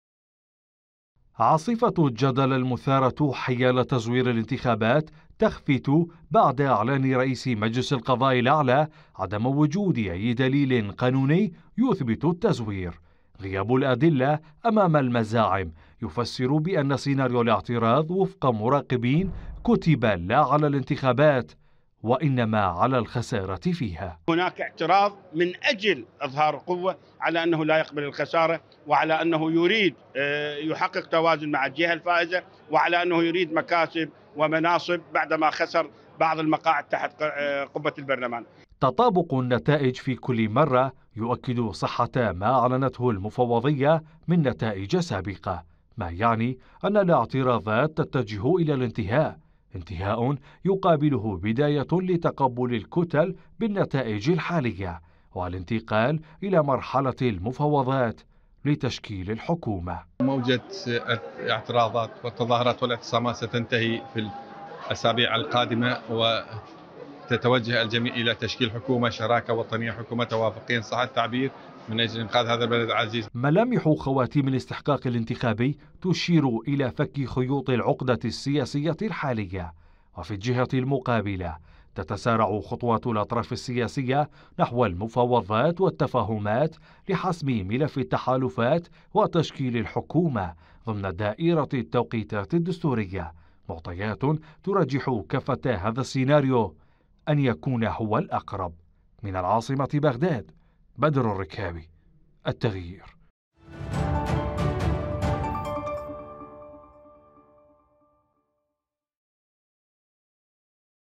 حقيقة تزوير انتخابات العراق تكشفها التغيير | تقرير
حقيقة تزوير_انتخابات_العراق تكشفها التغيير
وعلى الرغم من استمرار التظاهرات الرافضة لنتائج الانتخابات.. إلا ان الاعتراضات السياسية انخفضت حدتها بعد تأكيد مجلس القضاء الأعلى عدم وجود دليل يثبت التزوير, في حين رجح مراقبون انتهاء الأزمة قريبا والانتقال لمرحلة تشكيل الحكومة